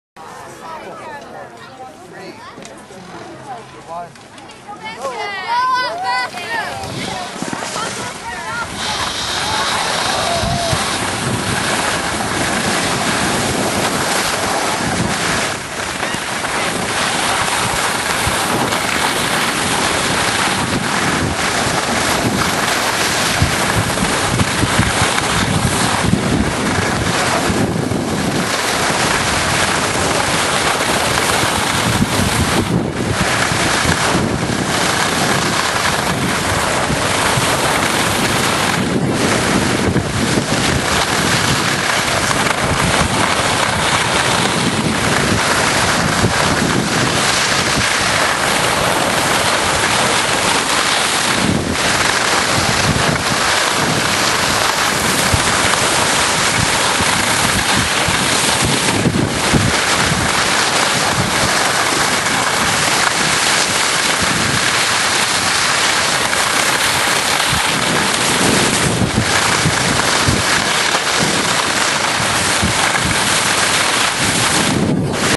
Bastian Skiing - 5 Years Old, First Ski Race, April 2009
Bastian skiing, 5 years old, first ski race, Alyeska, April 2009